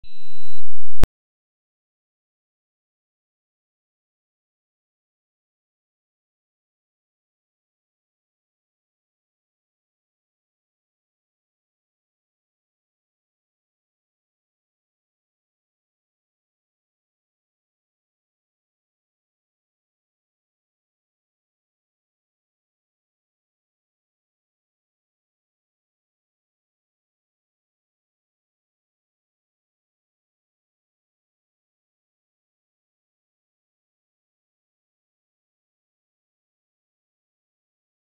Bike Theft in Laval, Montreal
Thieves stealing a DeVinci Tosca SL 1, caught on security camera.